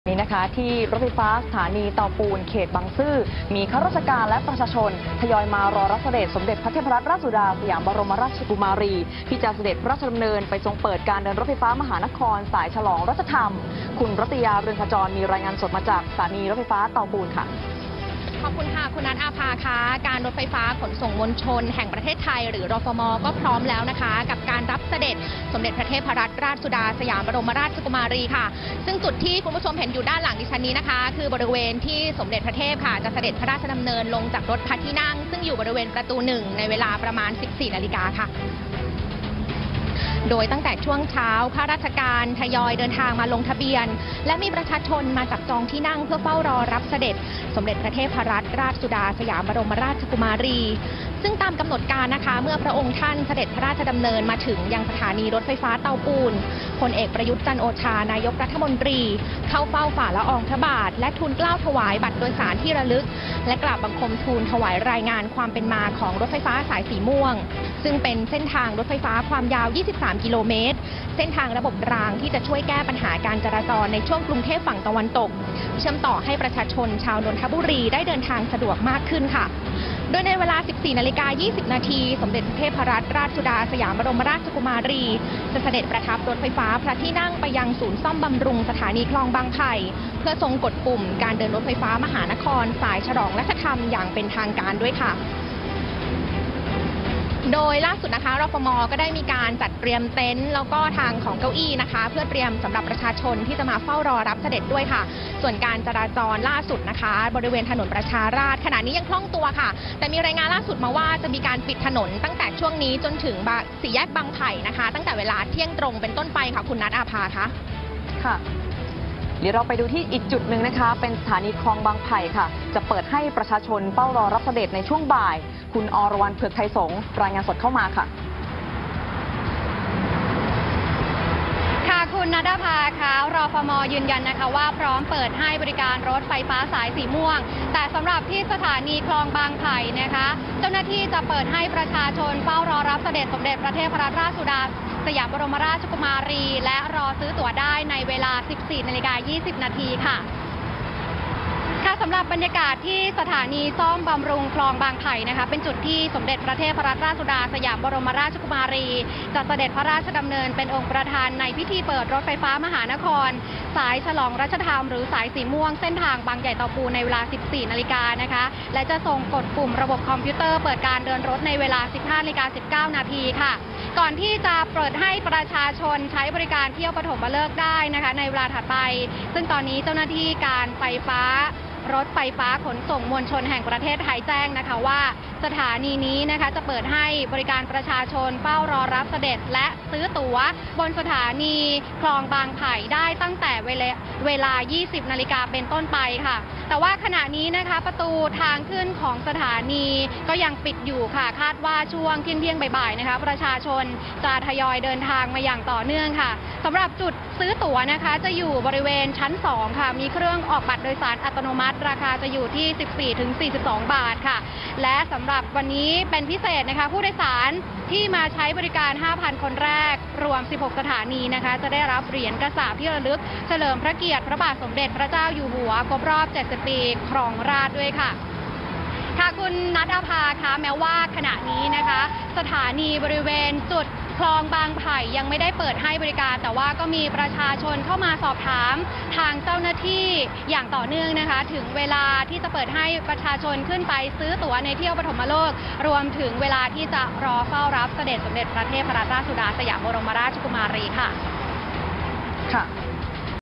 ประชาชนรอรับเสด็จสมเด็จพระเทพฯ เปิดรถไฟฟ้าสายสีม่วง
สถานีรถไฟฟ้าสถานีเตาปูน เขตบางซื่อ กทม. มีข้าราชการและประชาชนทยอยเดินทางไปรอรับเสด็จ สมเด็จพระเทพรัตนราชสุดาฯ สยามบรมราชกุมารี ที่จะเสด็จฯ ไปทรงเปิดการเดินรถไฟฟ้ามหานครสายฉลองรัชธรรม